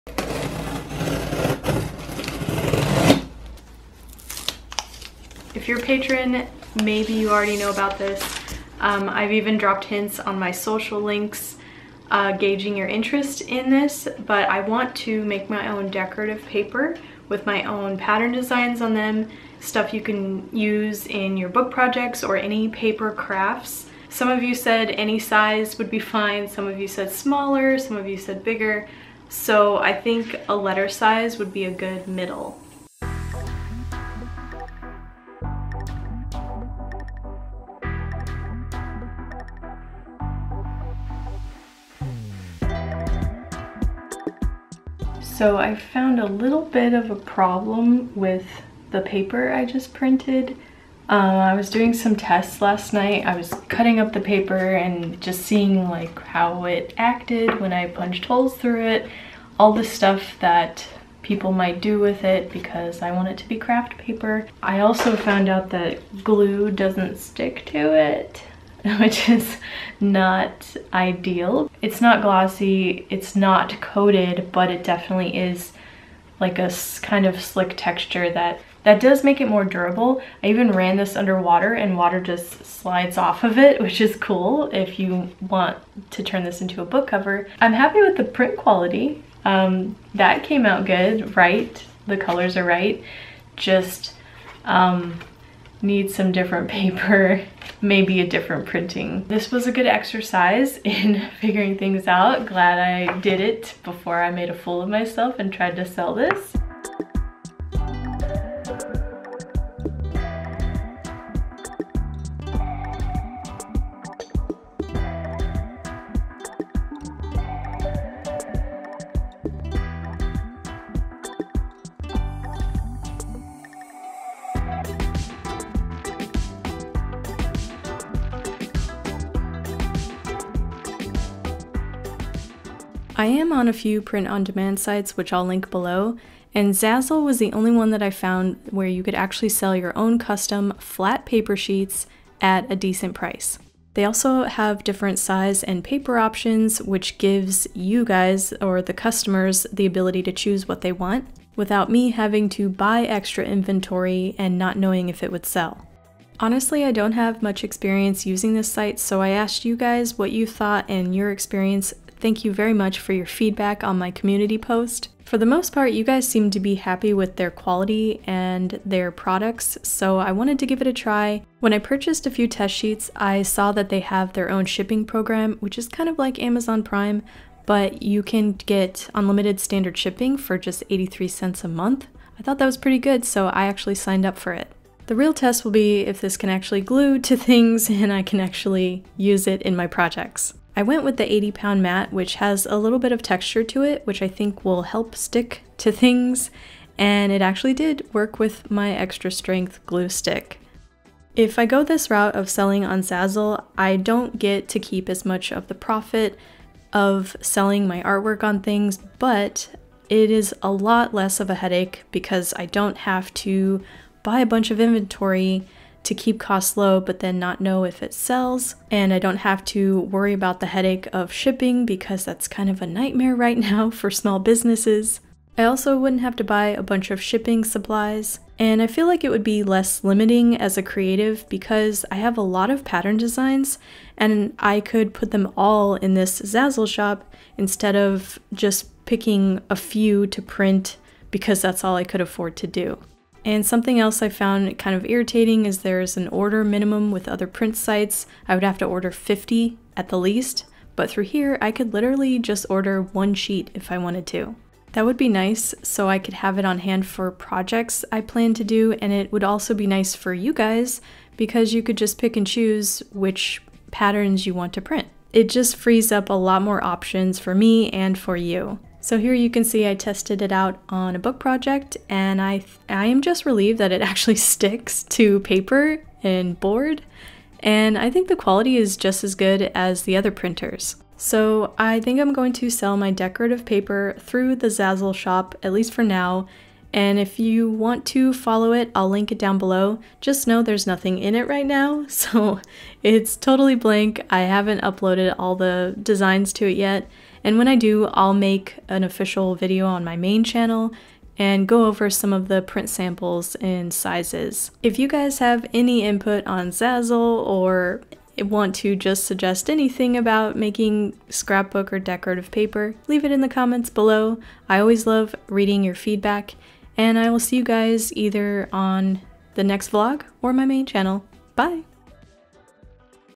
Paper Shouldn't Do This...
I like to design print patterns and I'm a bit of a paper addict—so naturally I want to make my own professionally printed scrapbook paper! Here's me figuring out how to do that.

MAIL
Sea Lemon
USA